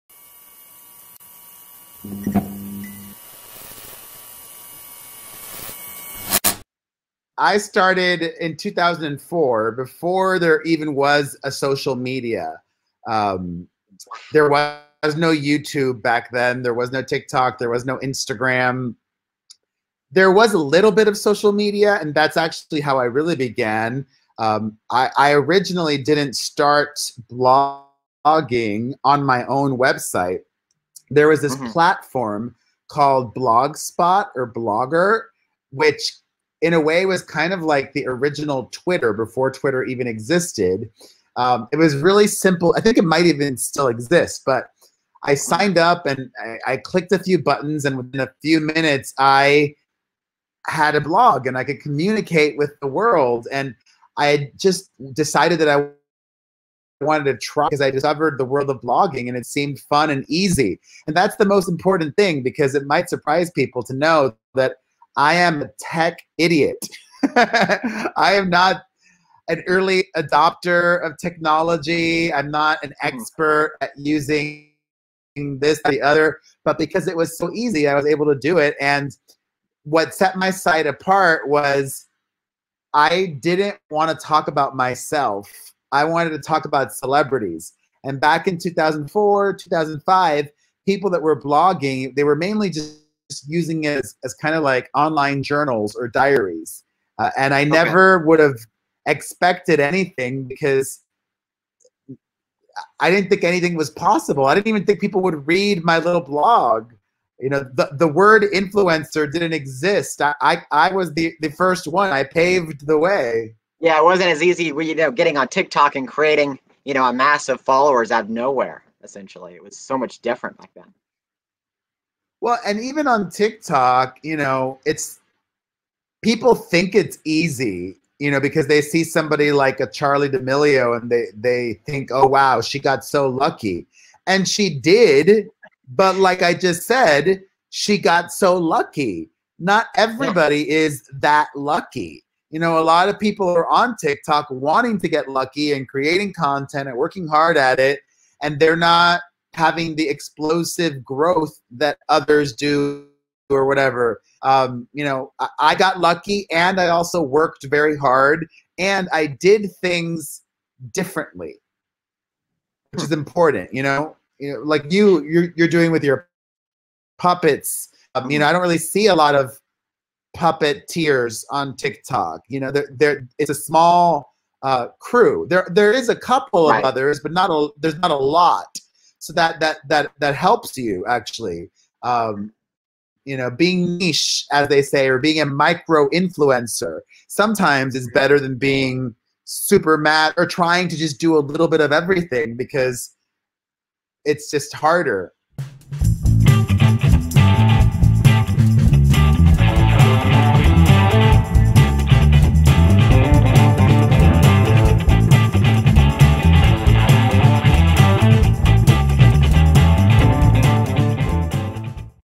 Landon LIVE! with Perez Hilton | How Perez Hilton became a Celebrity Blogger | LANDON HARVEY
Perez Hilton is an American blogger, columnist, and media personality. His blog, PerezHilton.com, is known for posts covering gossip items about celebrities. Hilton is also known for posting tabloid photos over which he has added his own captions or "doodles".

In this clip from a recent Landon LIVE! interview, Perez shares his story on how he became the blogger he is today!

Send Landon and The Gang Fan Mail:
7548 Preston Rd
Ste 141-1034